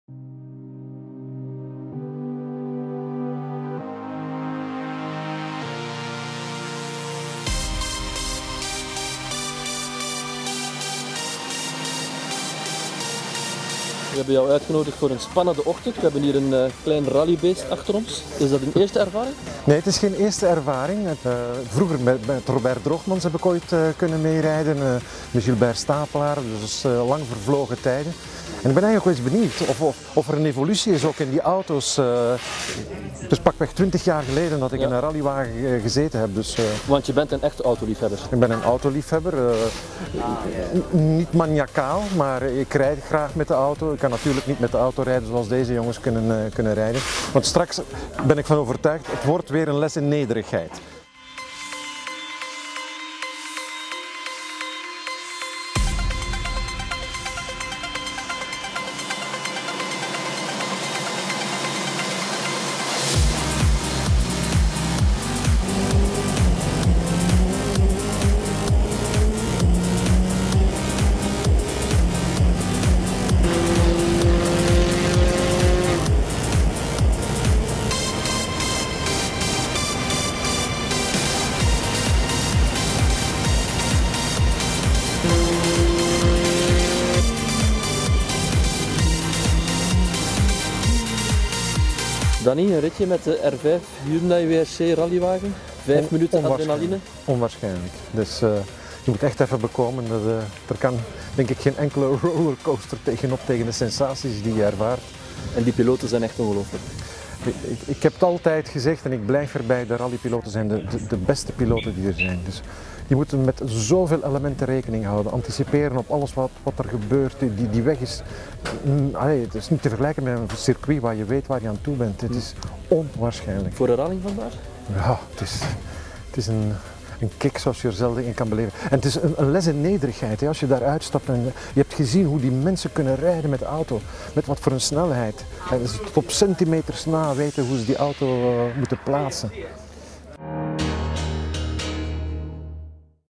Hyundai - Dany Verstraeten in de Hyundai i20 R5
Tijdens de Rally van Ieper debuteerde de nieuwe Hyundai i20 R5 rallywagen van Hyundai Motorsport. Wij vroegen nieuwslezer en autoliefhebber Dany Verstraten om als co-piloot de eerste kilometers mee te maken.